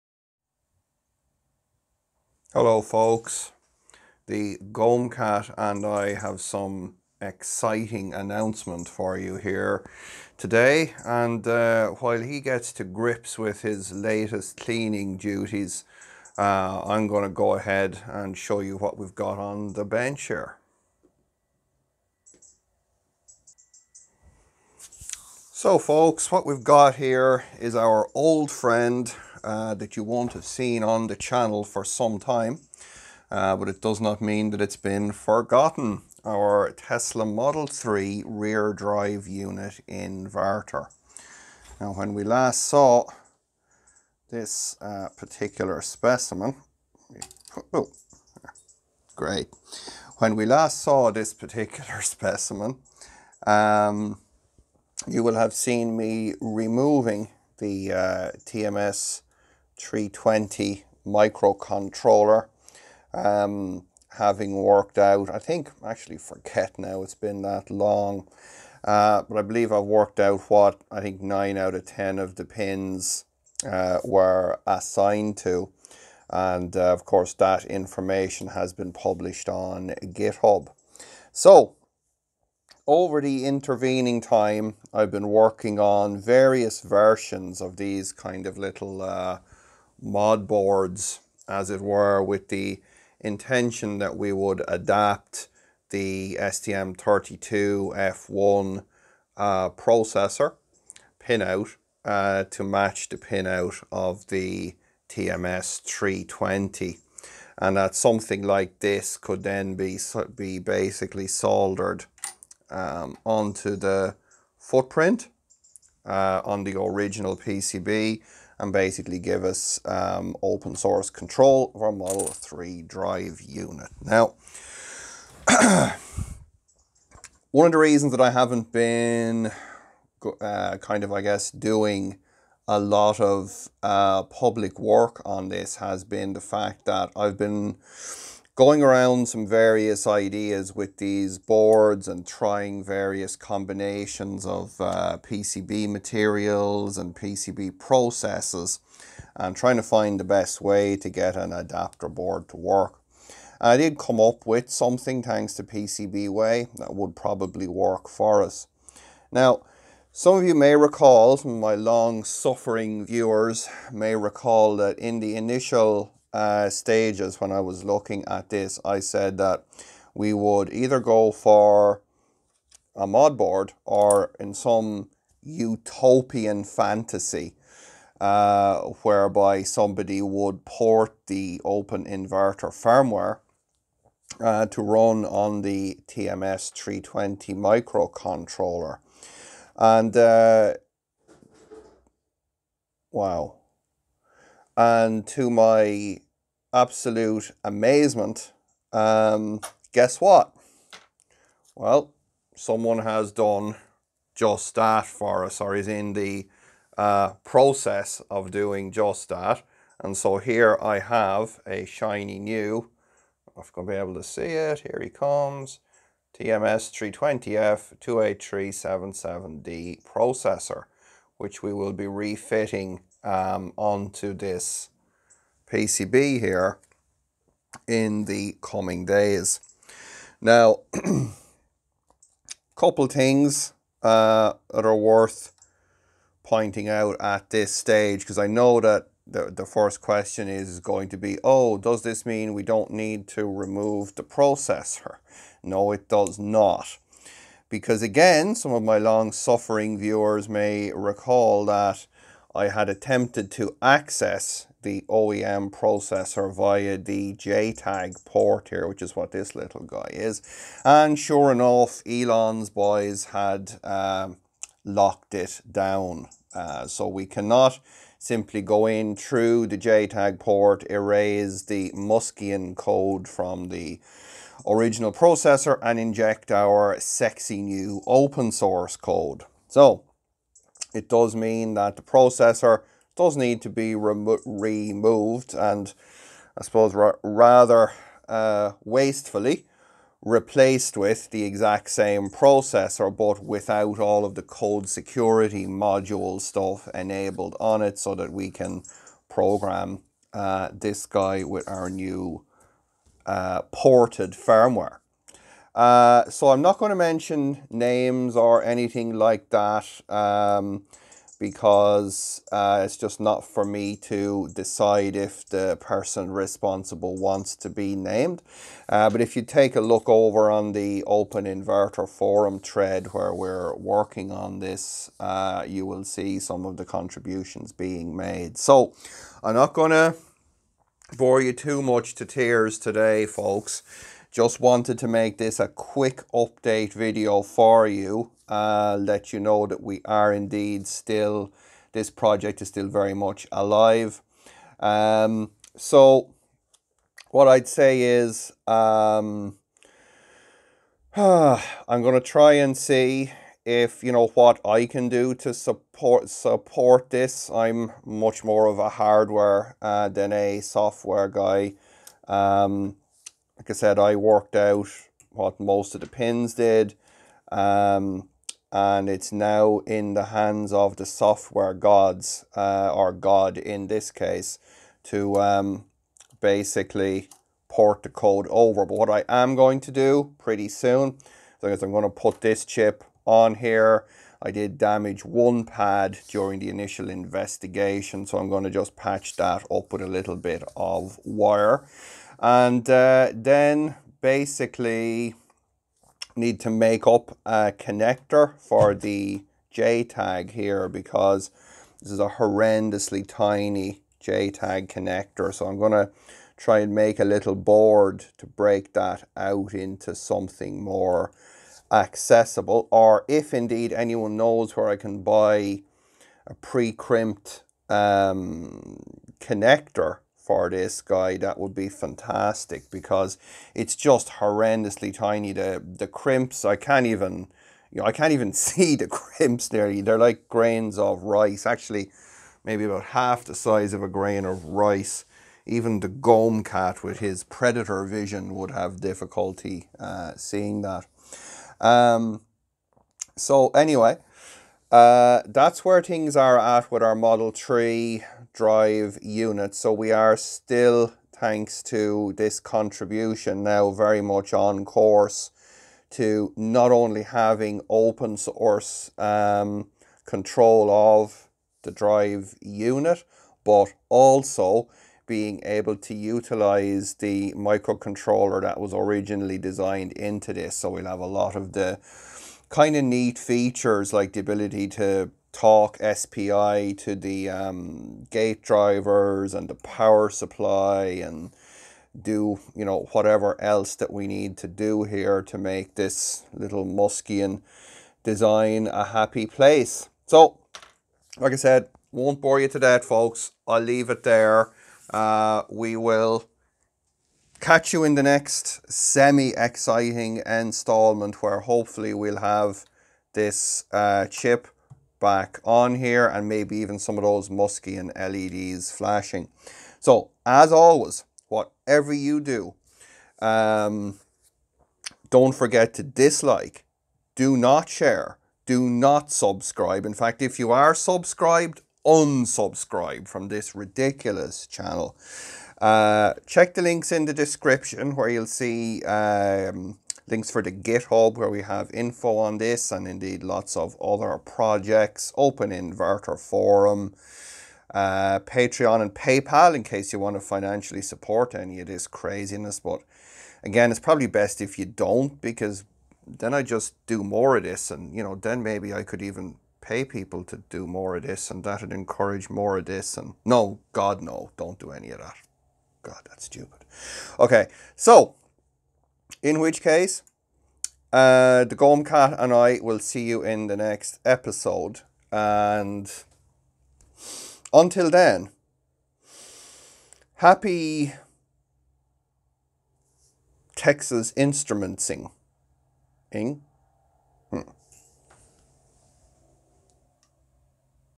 Tesla Model 3 Drive Unit Hacking Update
In this super boring episode we describe the new plan for opensource control of the Model 3 drive units.